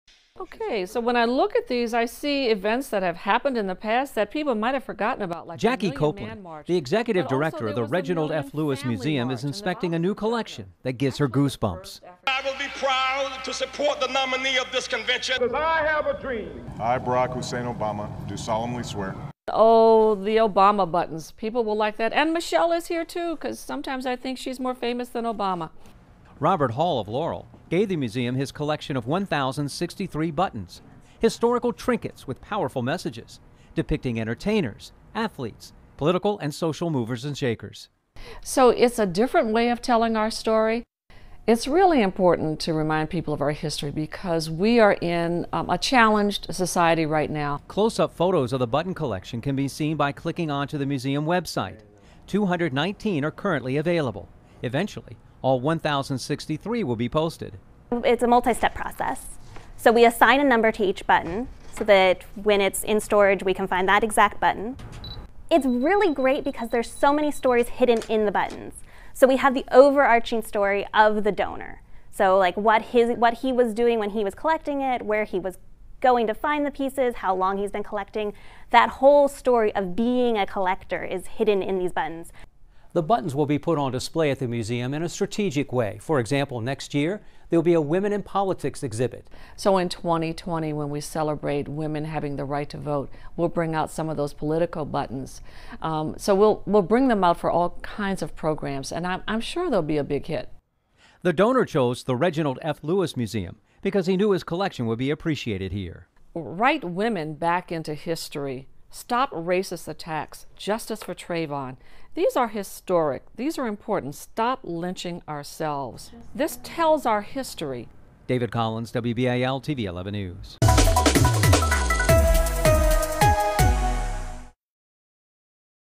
Over 1,000 buttons honoring African-American heritage donated to local museum
In a first of its kind for the Reginald F. Lewis Museum, a Maryland man donated more than 1,000 buttons he's been collecting for 30 years. They tell a story of African-American heritage, commemorating everything from entertainment to sports to politics.